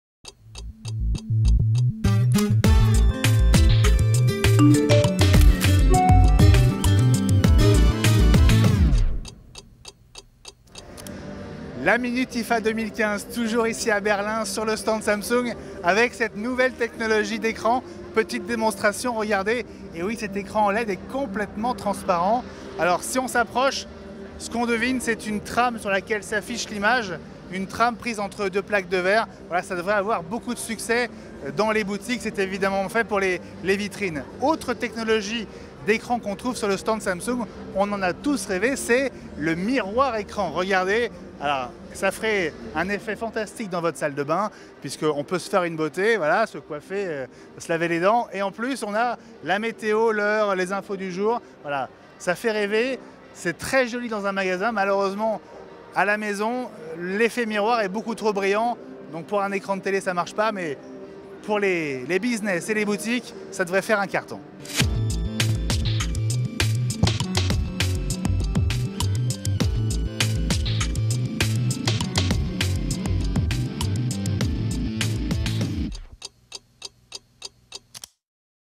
la minute IFA S03E02 : Transparent et miroir : les nouveaux écrans OLED Samsung
Benjamin Vincent, envoyé spécial de OUATCH TV à Berlin pour l'IFA 2015, vous présente deux nouvelles technologies d'écrans OLED développées par Samsung pour le secteur des magasins, boutiques, aéroports, etc : un écran OLED transparent et un écran OLED miroir.